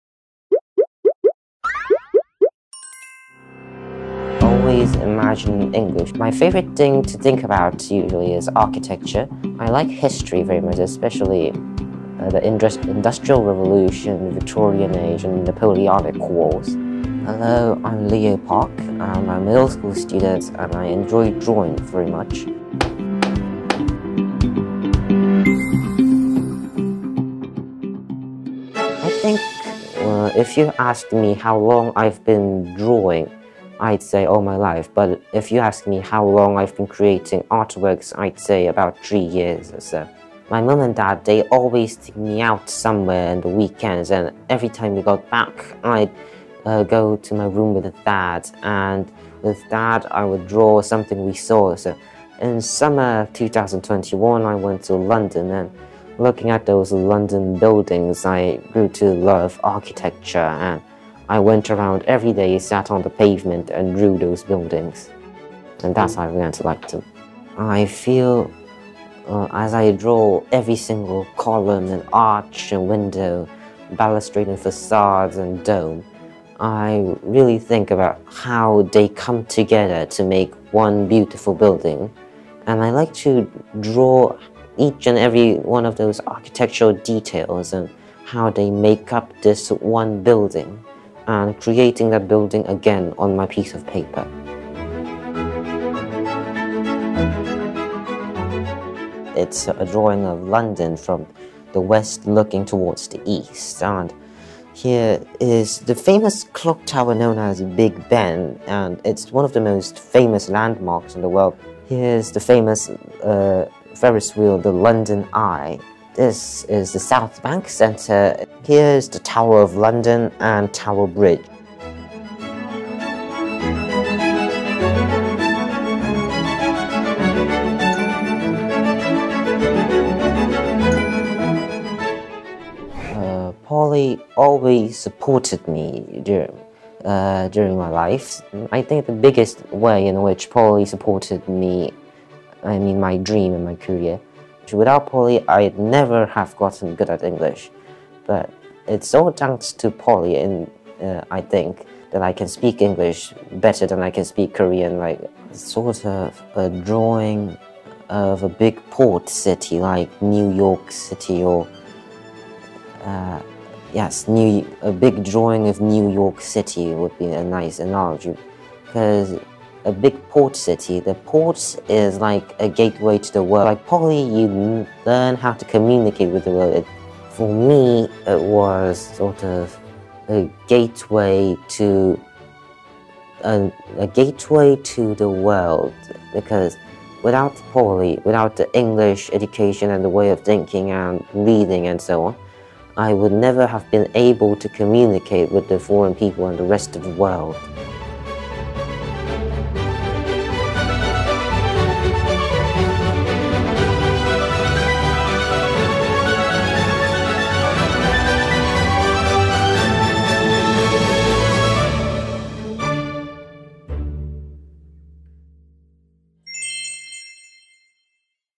[POLY Interview] 그림과 세상을 만나다_박소훈(Leo Park) 학생
아름다운 세계의 건축물과 도시 풍광을
자신만의 시선으로 담아낸 그림을 통해
전 세계 사람들과 소통하고 있는 13세 천재 화가, Leo
그가 꿈과 자신감을 키우고
세상을 향해 한 발 내디딜 수 있도록 용기를 준 지지자는 과연 누구?
더 큰 꿈을 펼쳐주는 곳, POLY에서
Leo의 이야기를 만나보세요.